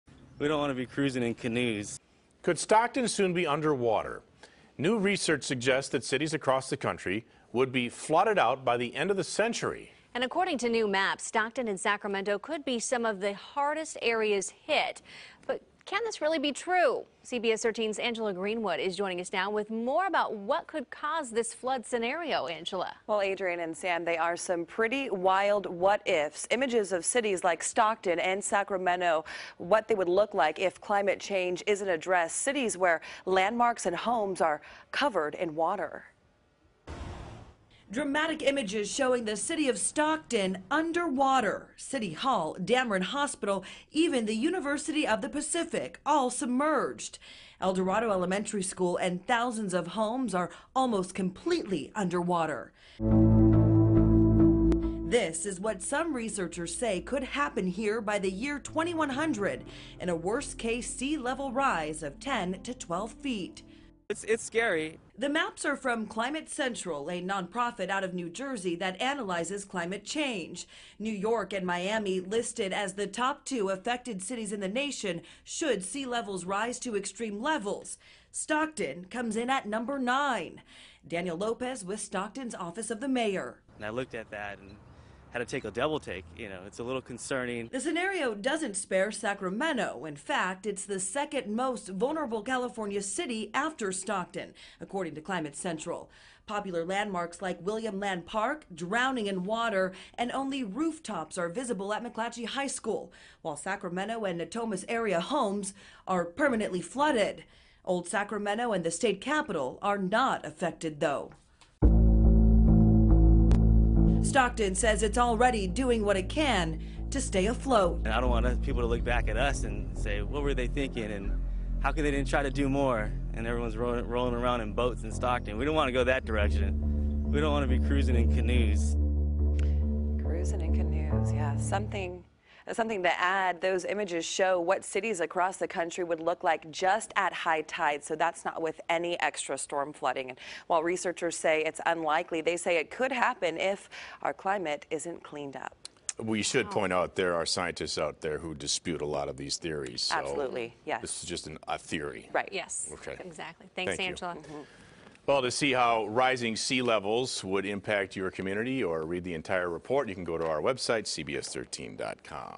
Research Shows Impact Of Rising Sea Levels On Stockton, Sacramento
Stockton City Hall, Dameron Hospital, even the University of the Pacific are all submerged. El Dorado Elementary School, along with thousands of homes are almost completely under water.